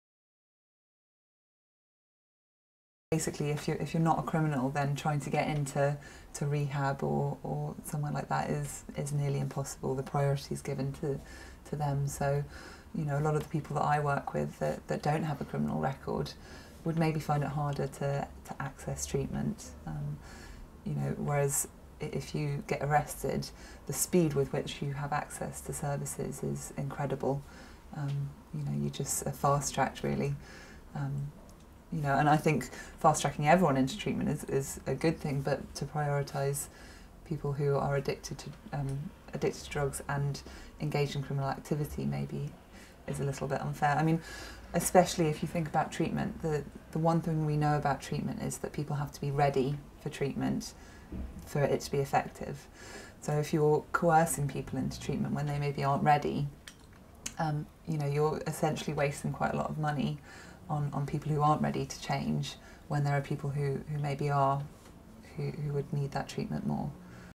7/18 People missing out on treatment due to current drug policy
Dr Katharine May is a psychologist who works in addiction.  She wrote her Masters on substance misuse.  It was called 'The Myth of the Addict'.  She discusses this and other aspects of drug use, misuse and policy in this interview.